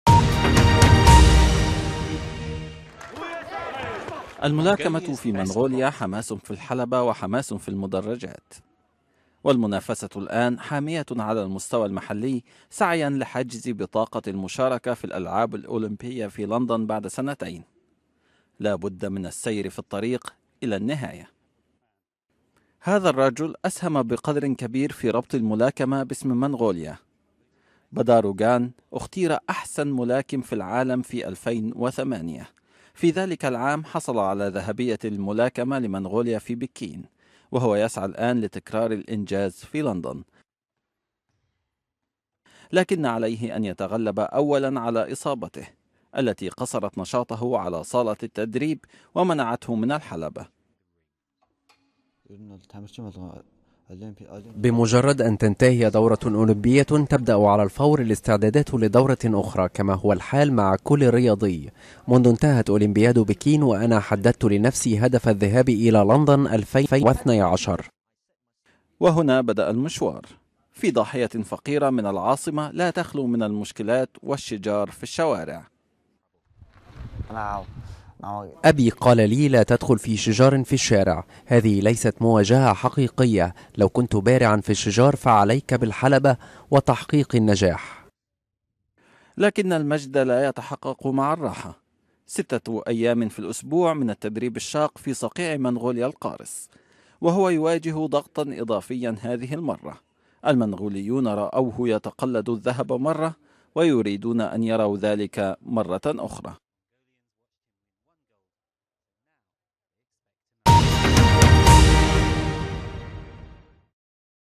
بادار اوغان من أطراف المدينة إلى مركز الضوء
سنتان بالتمام والكمال وتنطلق الألعاب الأولمبية في لندن في 2012، وأبطال العالم يستعدون لرحلة إلى شرق لندن يحلمون فيها بالذهب.